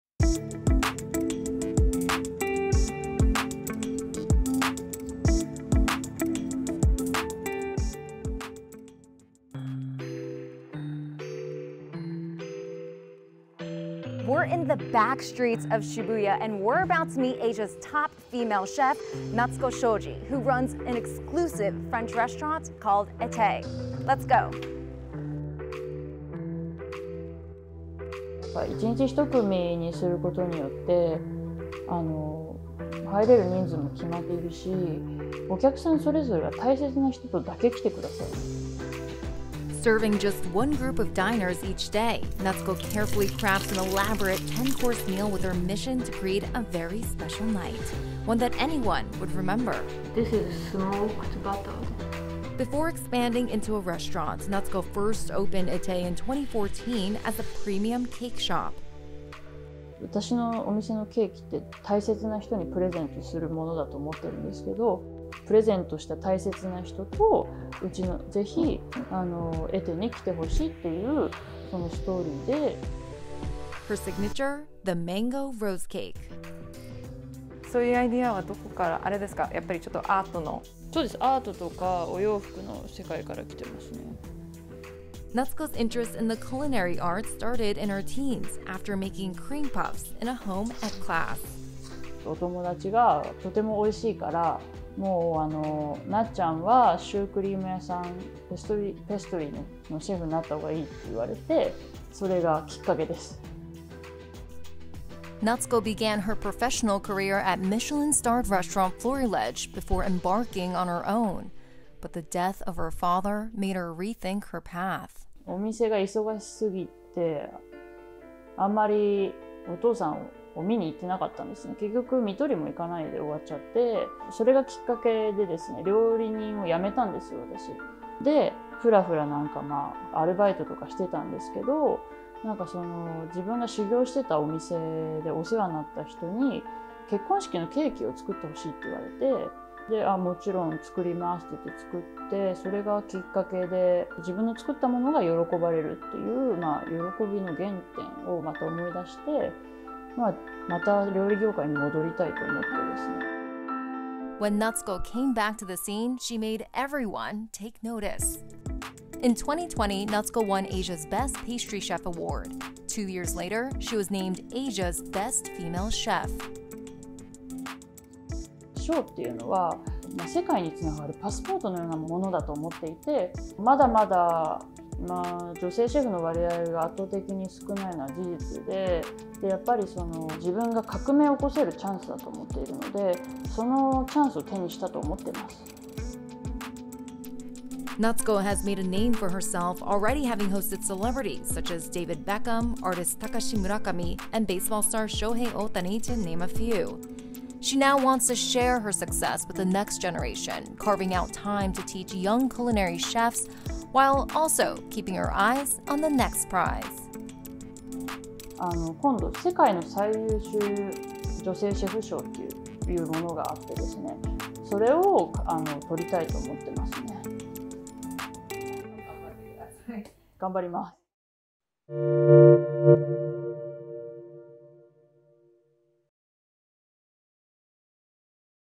Inside the Business of Asia’s Top Female Chef Natsuko Shoji
Asia’s top female chef and businesswoman Natsuko Shoji makes artistic cakes that cost up to $2,000 each. Natsuko first opened été in 2014 as a premium cake shop, selling luxurious desserts featuring premium Japanese fruit. Having trained at Michelin-starred restaurant Florilege in Tokyo, she expanded the cake shop into an upscale French restaurant. This is "Her Story."